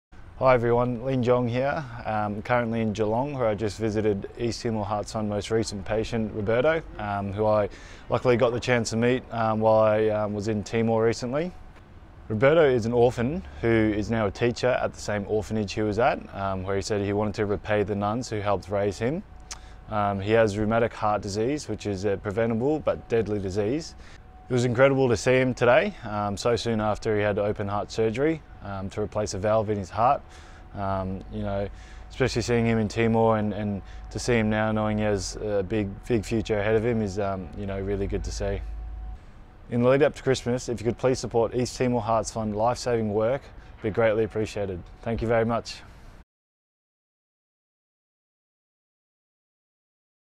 Lin Jong visits patient Roberto
Patient Roberto became a teacher at the orphanage where he was raised, to help repay the kindness of the nuns who cared for him. But his heart disease left him too sick to teach. After his heart surgery at University Hospital Geelong this month, Roberto is feeling  great and will soon be back in the classroom. Our ambassador,  Western Bulldogs champion Lin Jong, paid a visit to wish Roberto well.